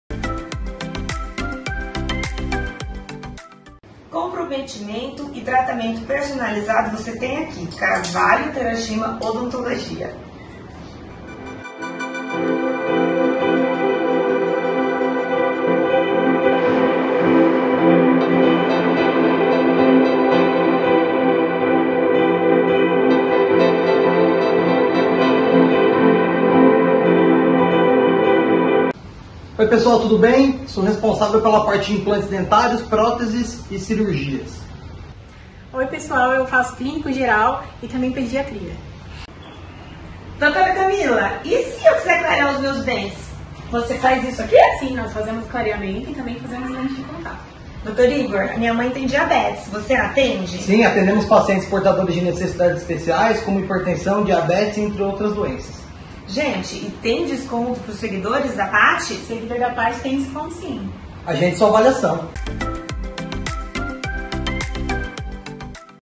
Uma dica para melhorar o seu sorriso
Pat Almeida apresenta a Carvalho Terashima, em Mogi das Cruzes. Confira!